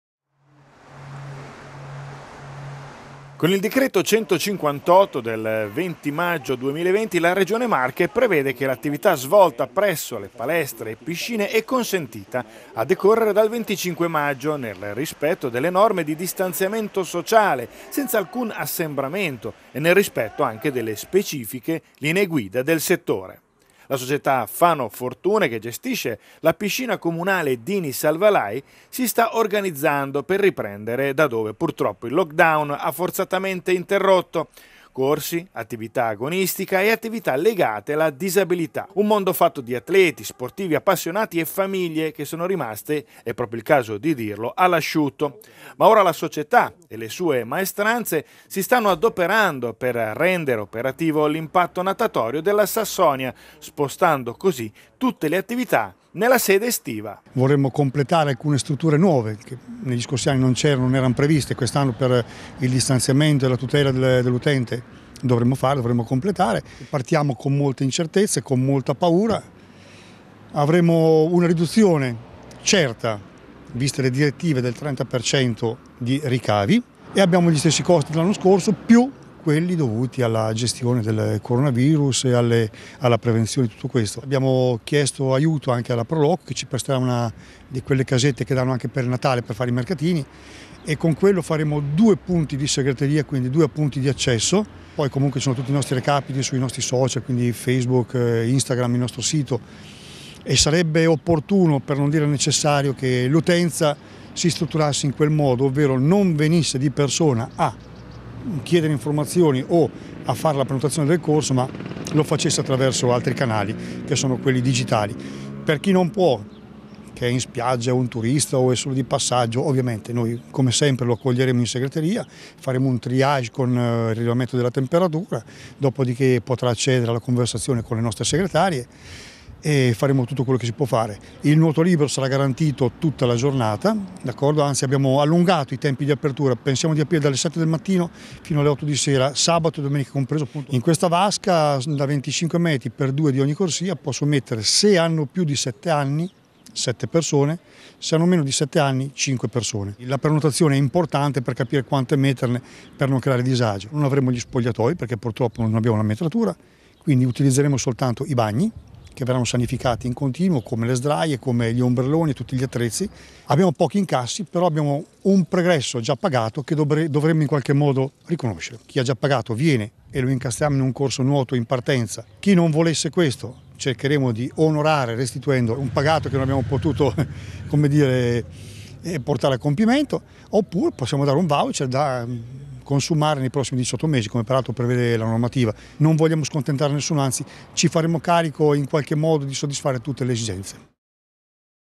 Piscina Sassonia, tutto pronto per accogliere i nuotatori
Per la gioia di agonisti e frequentatori, ancora pochi giorni e la piscina della Sassonia sarà fruibile sempre ovviamente nel rispetto delle disposizioni regionale e di distanziamento fisico.